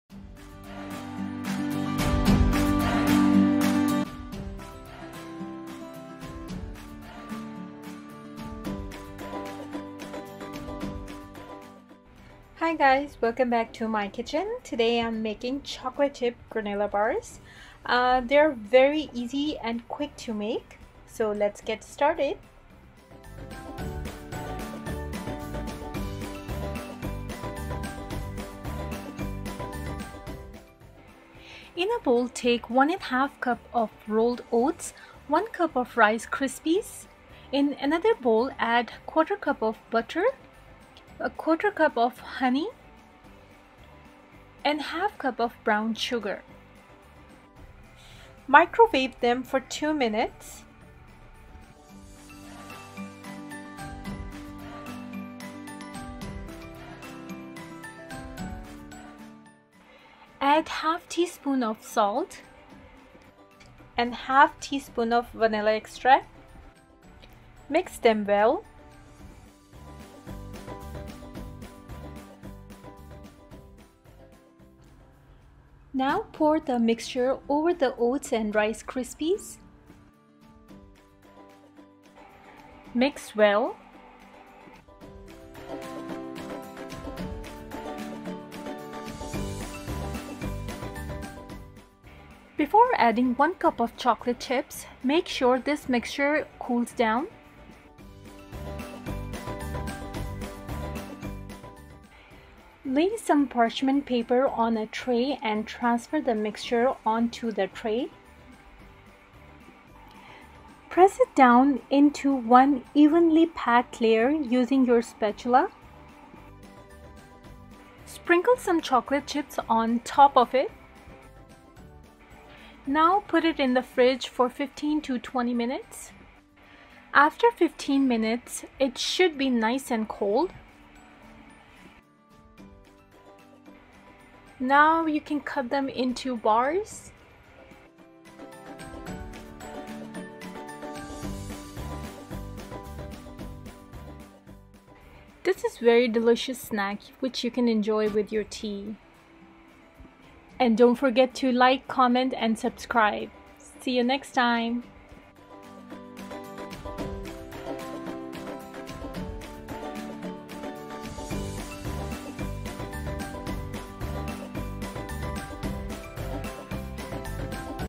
Chocolate Chip Granola bars by Desi Food Fusion I The easiest granola bars you'll ever make
The easiest granola bars you'll ever make! Made with oats and rice cereal

Ingredients:
Rolled Oats                    1 1/2 Cup
Rice crispy cereal         1 cup of
Chocolate chips.          1 Cup